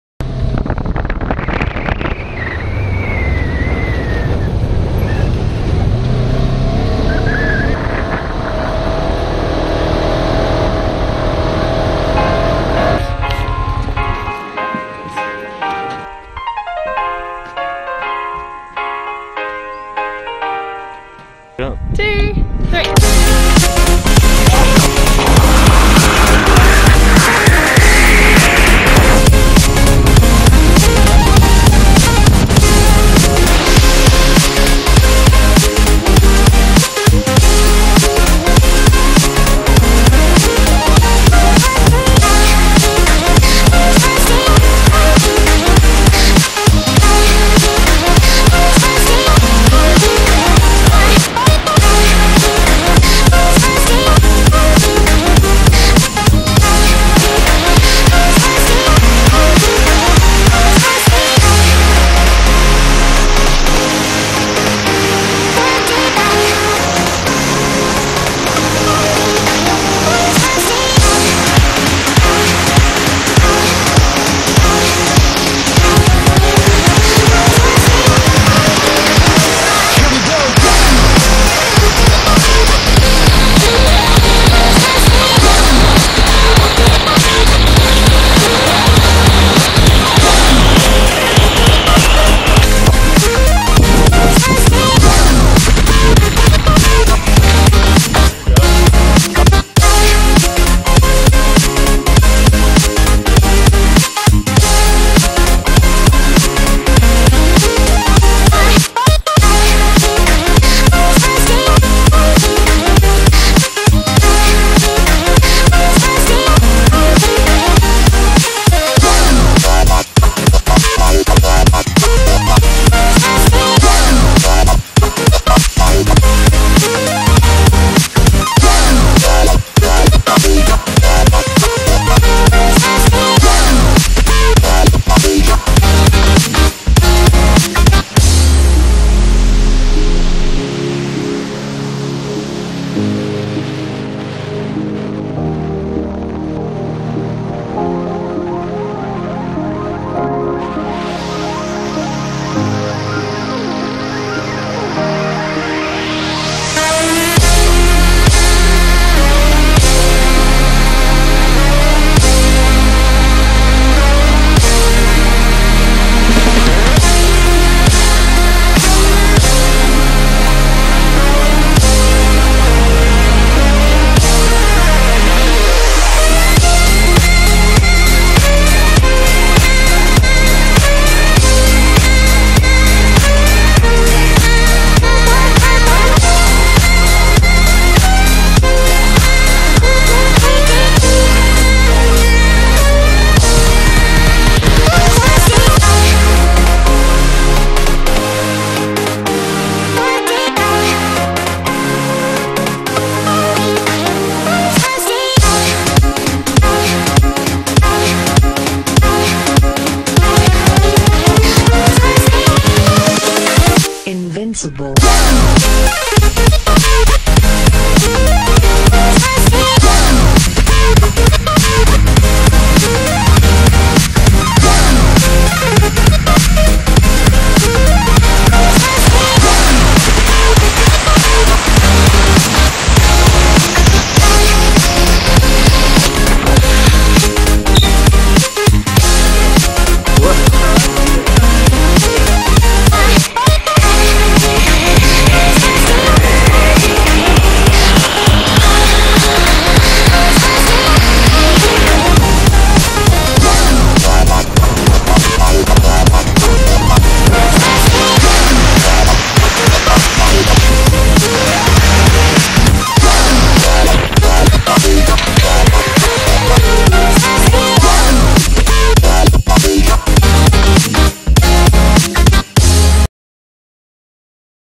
Superfly Whistler - 4 of the longest, fastest and highest ziplines in Canada
riding all four ziplines at whistlers Superfly zipline